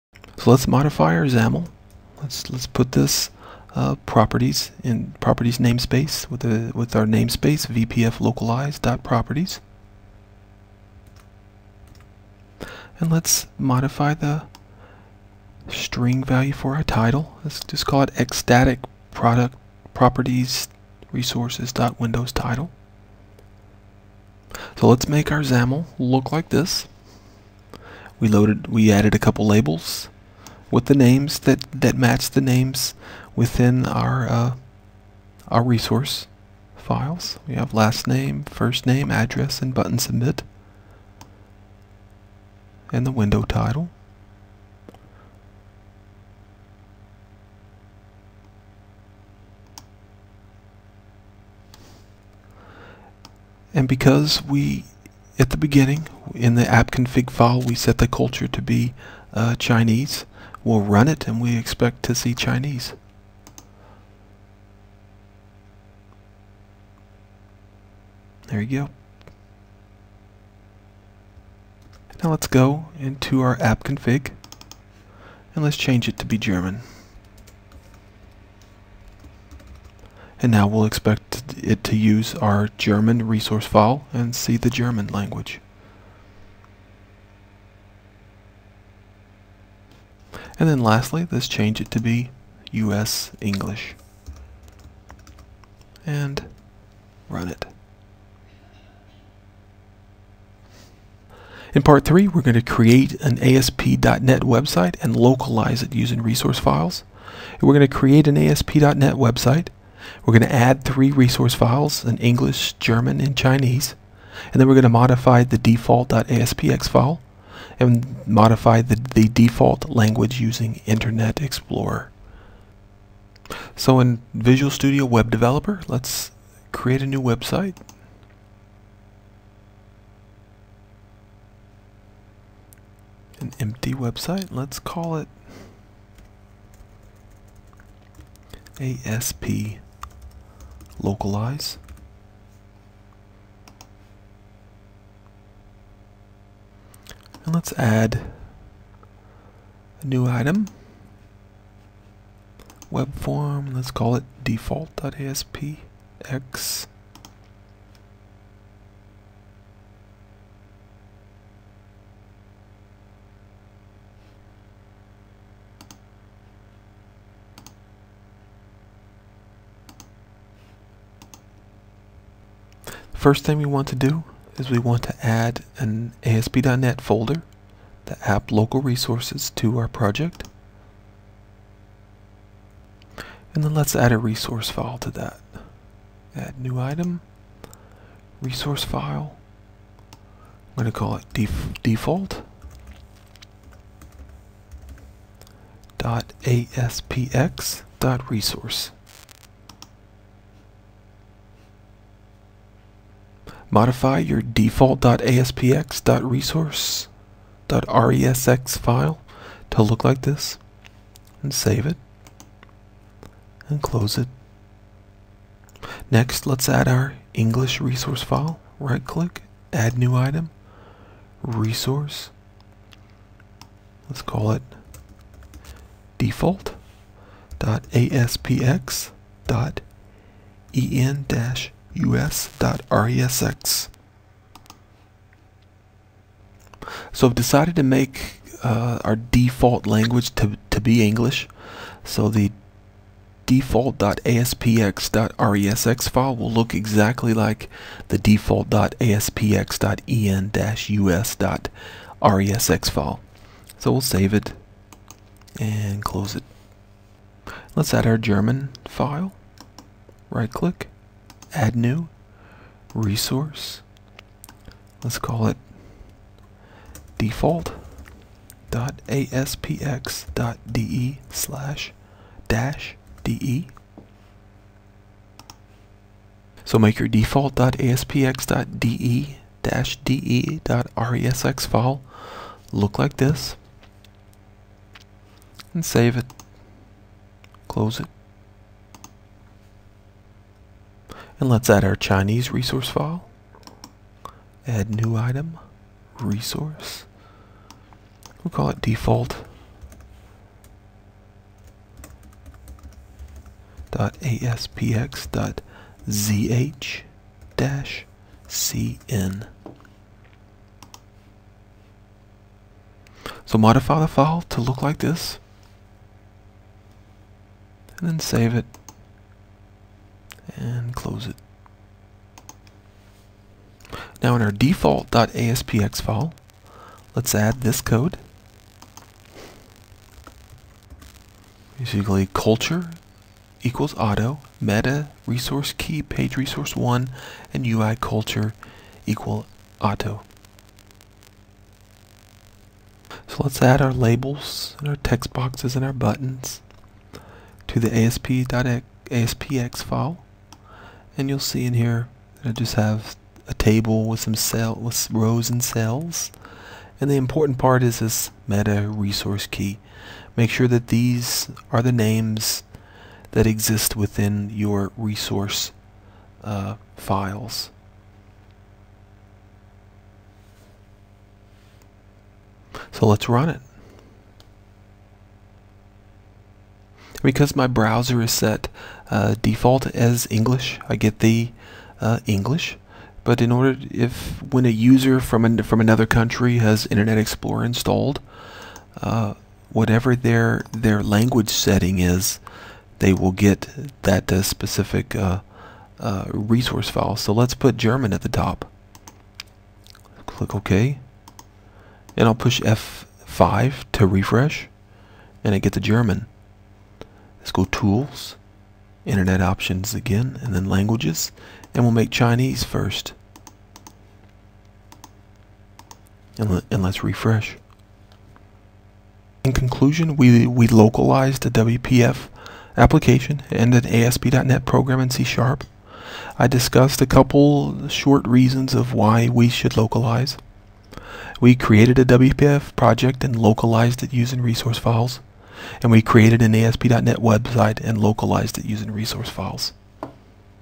Lesson 12 (Part 2) - Localize WPF and ASP.NET using C# and XAML
In Lesson 12 you will learn to:
Localize a WPF application using resource files, XAML and C#.
Localize an ASP.NET application using resource files.